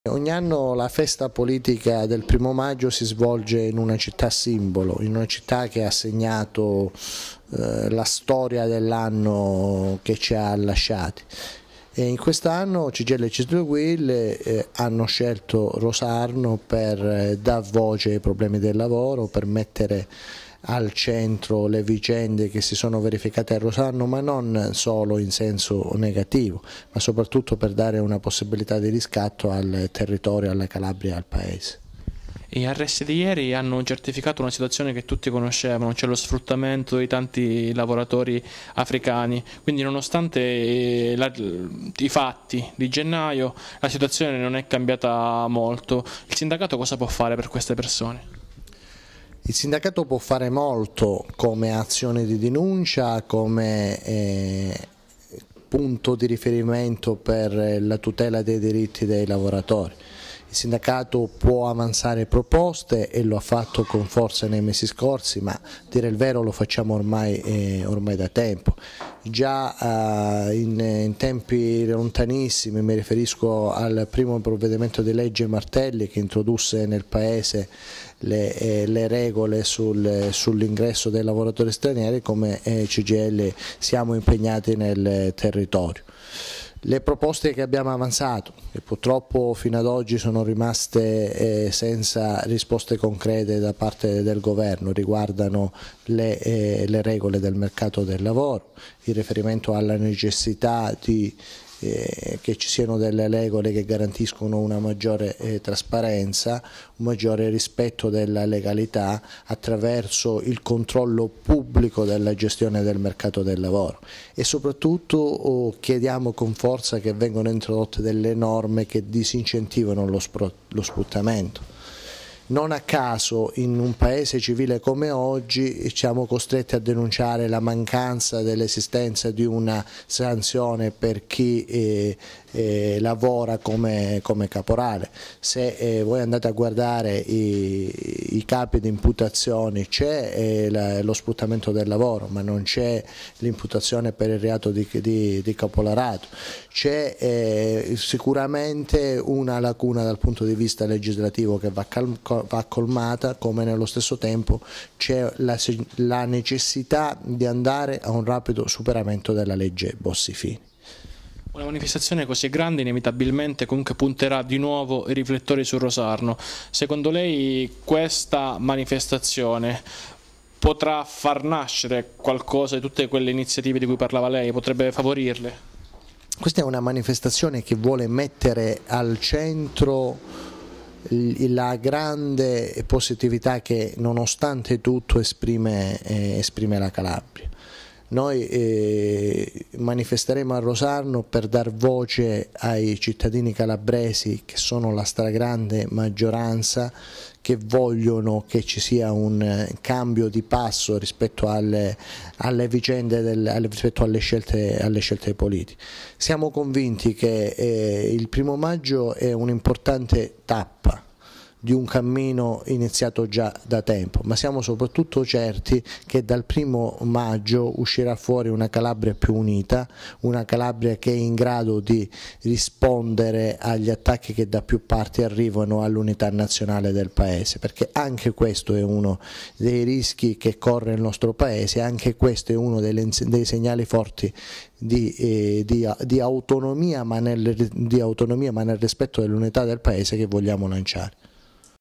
Antonino Calogero sul primo maggio a Rosarno
di Lucio Rodinò
InquietoTv: Antonino Calogero, segretario generale della Cgil della Piana di Gioia Tauro parla della manifestazione nazionale organizzata da Cgil, Cisl e Uil che si terrà il primo maggio a Rosarno.